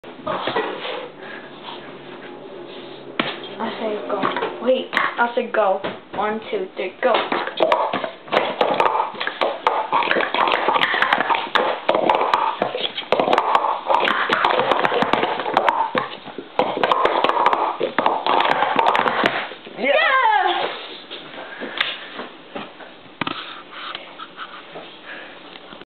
Sport Stacking : Double in 12.33 sec.
I teamed up with Steven Wu for a Doubles run. Turn out, a new record.